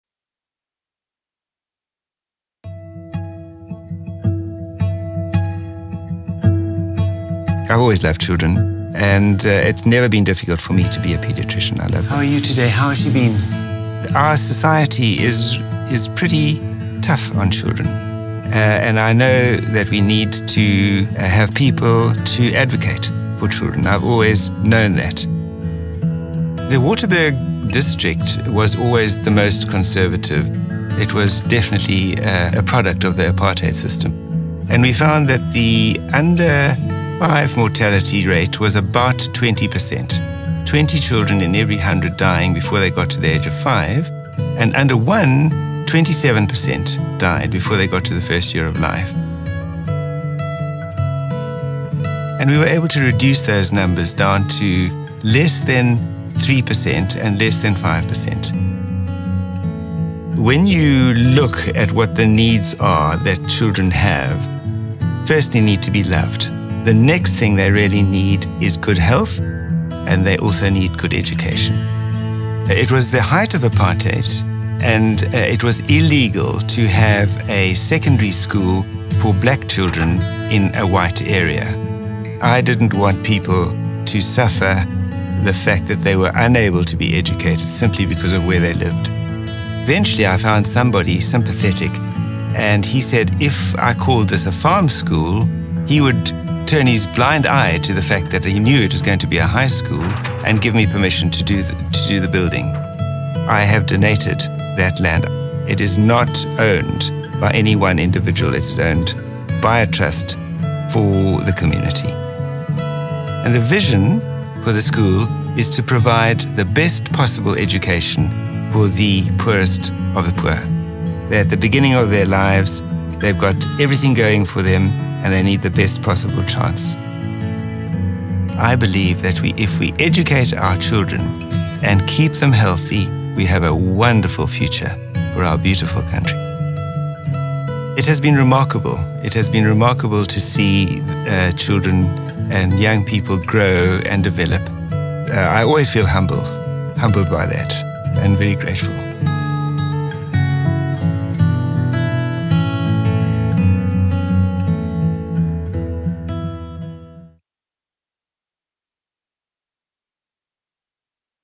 Dr Peter John Farrant - 2012 Inyathelo Award for Lifetime Philanthropy
Farmer and doctor Peter John Farrant received the 2012 Inyathelo Award for Lifetime Philanthropy for his contribution to secondary education and the prevention and treatment of HIV.